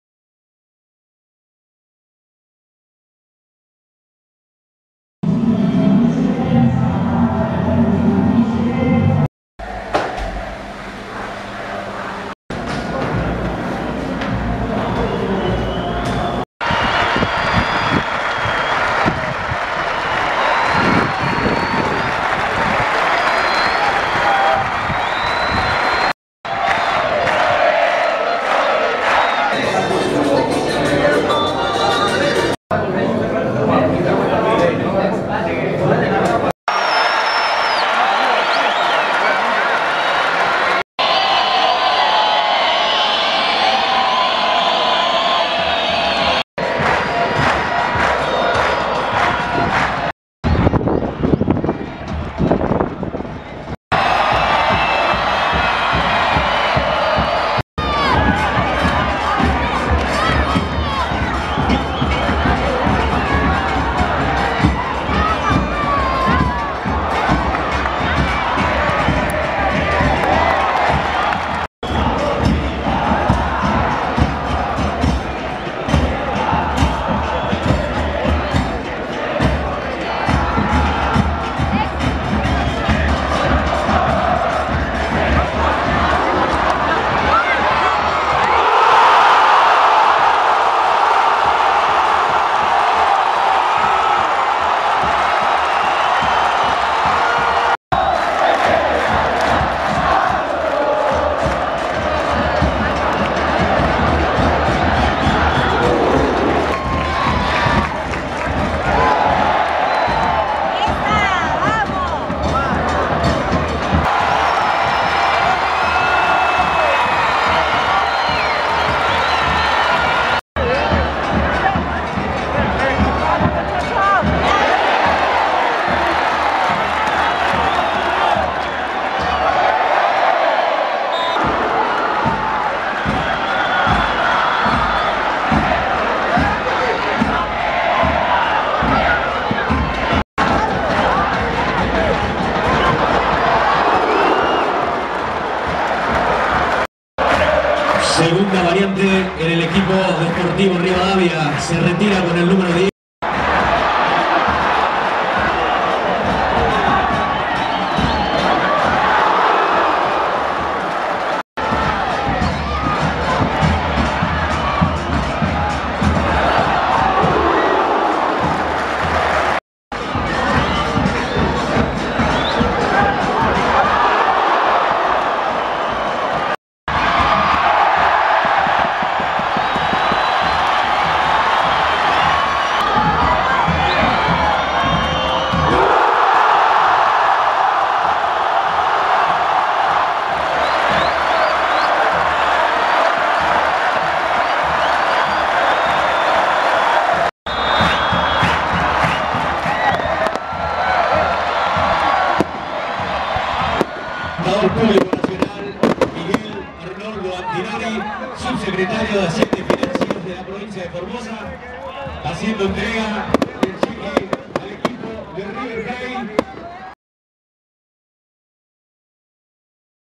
Los mejores momentos de River 3 - Sportivo Rivadavia (VT) 0
Lucas Alario, Ignacio Fernández y Gabriel Mercado marcaron los tantos del encuentro de 32avos disputado en el Antonio Romero.